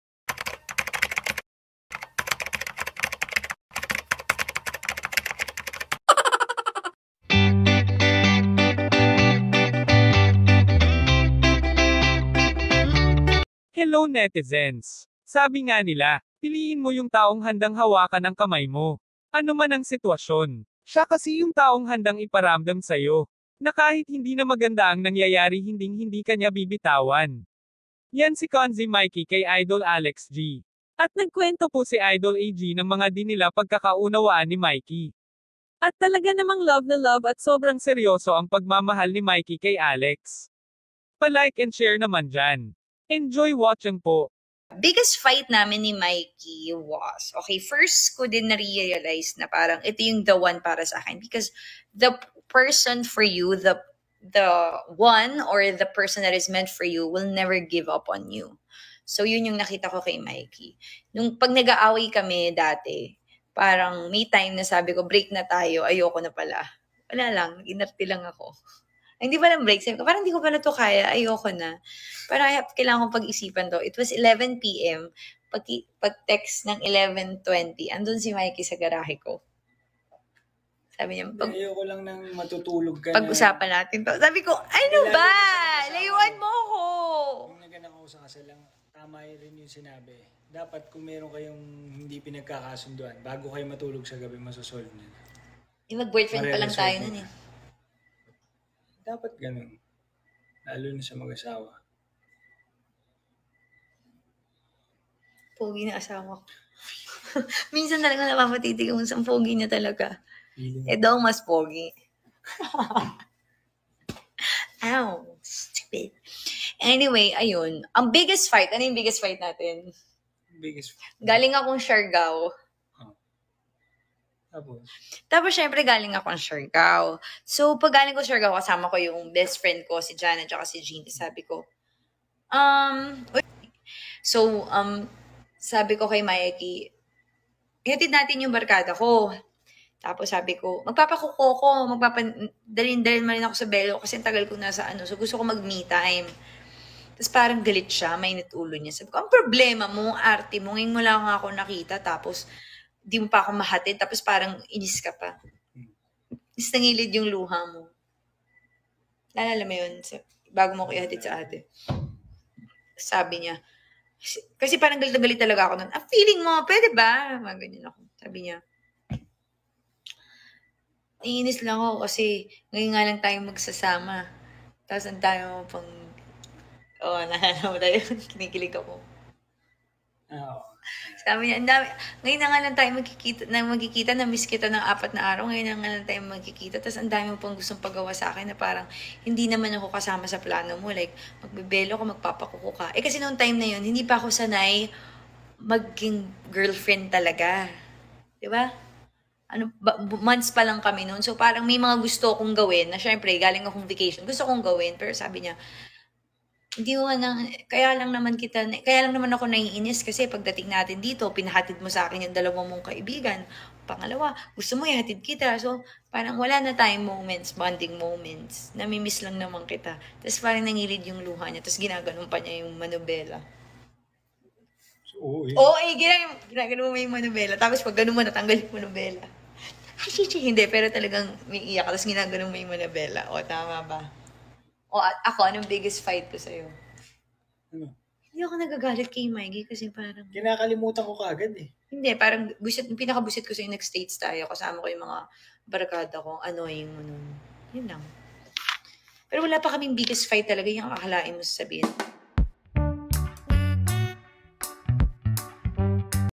KONSI MIKEE MORADA GAGAWIN ANG LAHAT PARA KAY ALEX GONZAGA! AG SUPER KILIG SA MISTER!
AG & Mikee, naka support kami lagi sayo!❤️

Isaiah 60:22
"When The Time Is Right
I,The Lord Will Make It Happen" ❤️

lyka: cathygonzagamorada

See you on my next vid!
XOXO, POWERTV ❤️